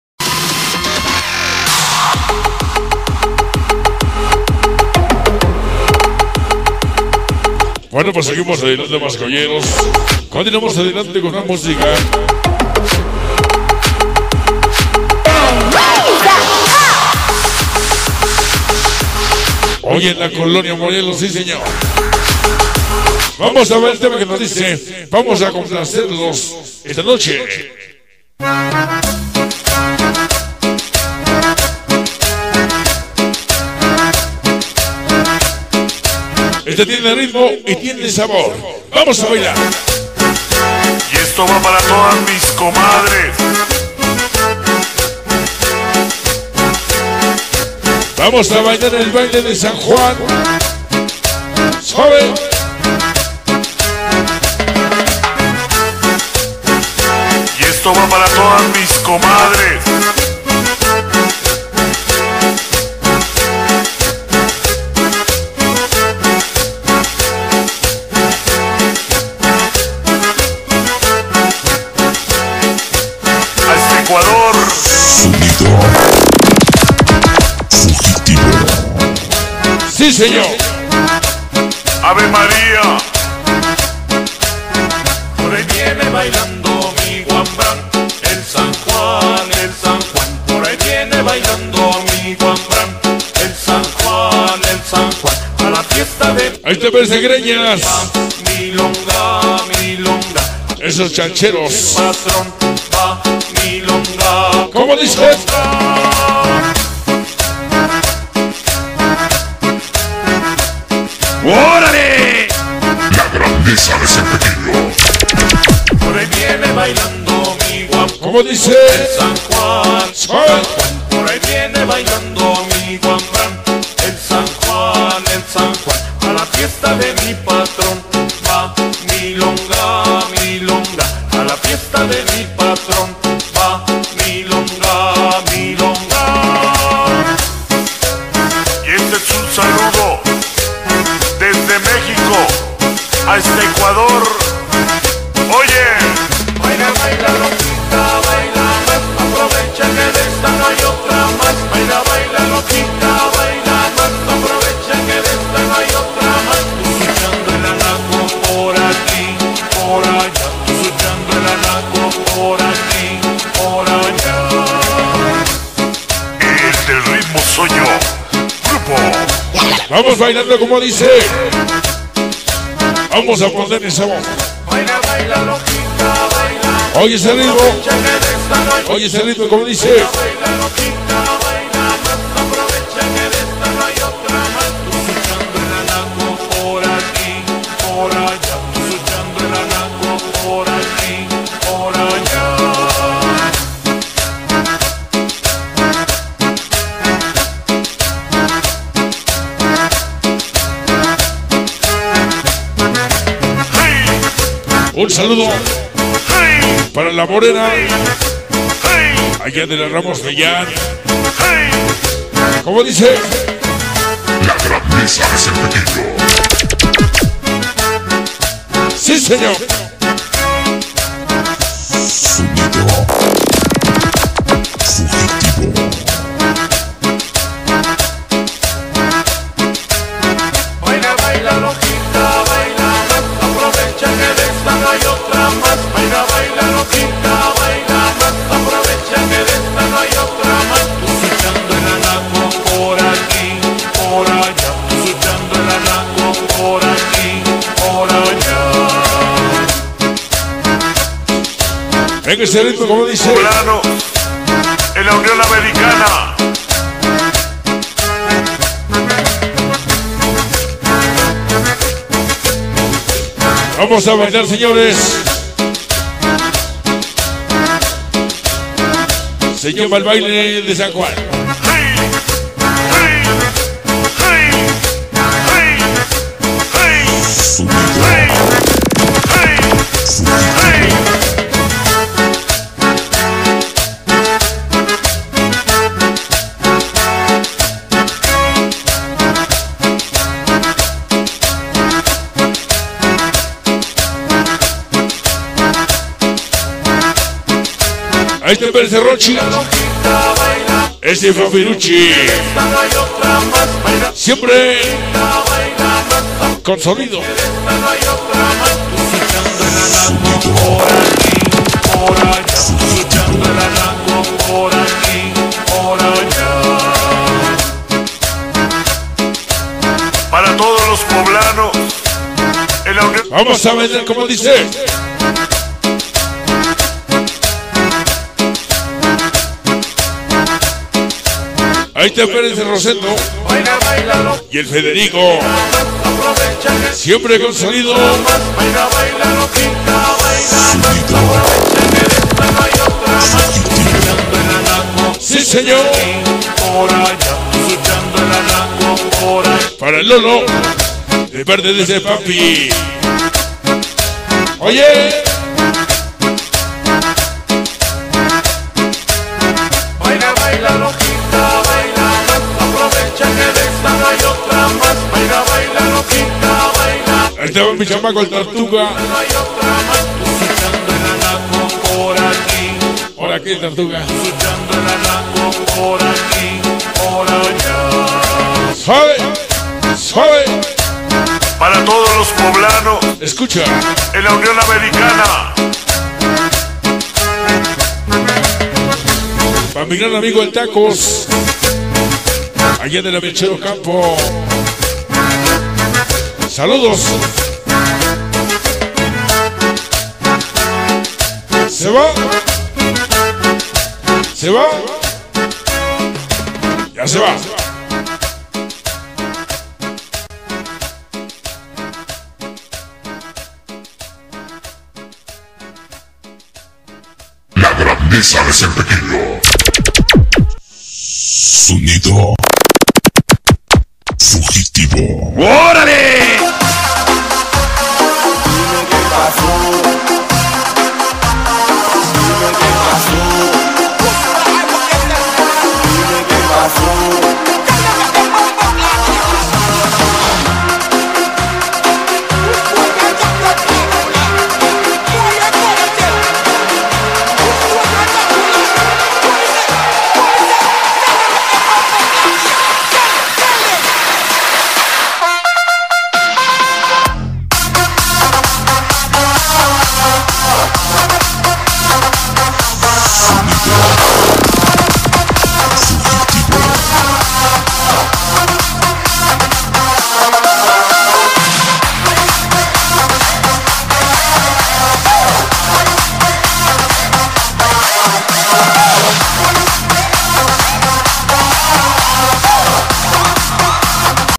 sonido fugitivo baile de san juan col morelos vol 1 6
sonido fugitivo ixtapaluca edo de mex colonia morelos 4 aniversario al santo patron san judas tadeo 28 de octubre del 2015